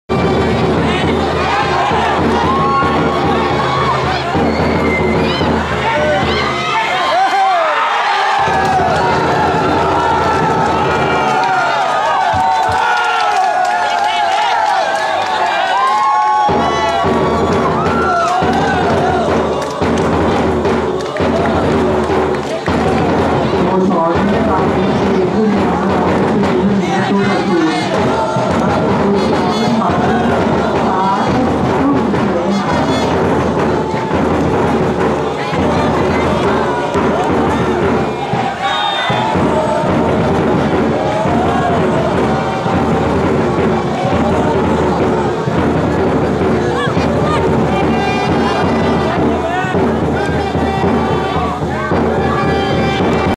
พะยูนบุกขย้ำฉลามอันดามัน 1-0 (สาธิต)
จบครึ่งแรกระนอง 0-1 ตรัง เอฟซี (สาธิตนาทีที่ 44)